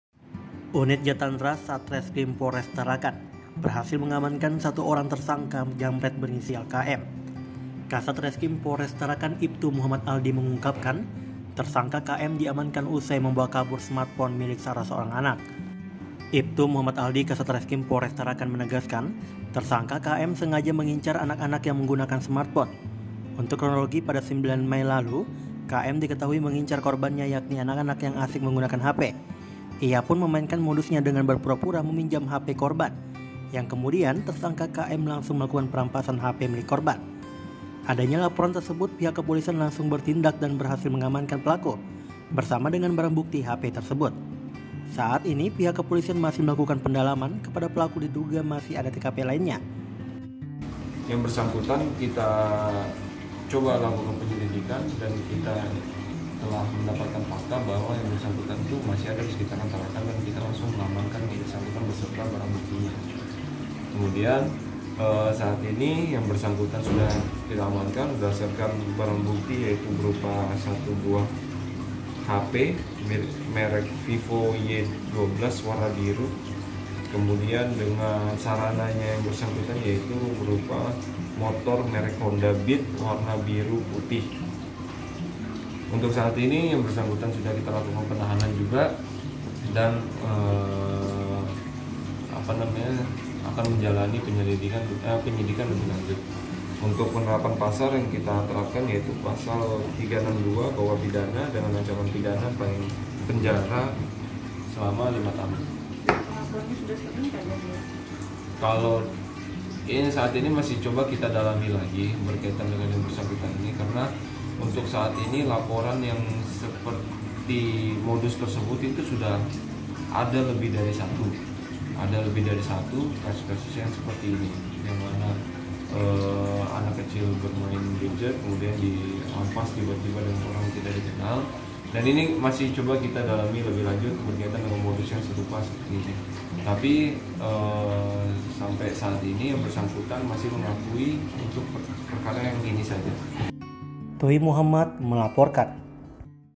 MODUS PINJAM, HP KORBAN DIBAWA LARI
JAMBRET MENYASAR ANAK-ANAK.3026

UNIT JATANRAS SATRESKRIM POLRES TARAKAN BERHASIL MENGAMANKAN SATU ORANG TERSANGKA JAMBRET BERINISIAL KM, KASAT RESKRIM POLRES TARAKAN IPTU MUHAMMAD ALDI MENGUNGKAPKAN TERSANGKA KM DIAMANKAN USAI MEMBAWA KABUR SMART PHONE MILIK SALAH SEORANG ANAK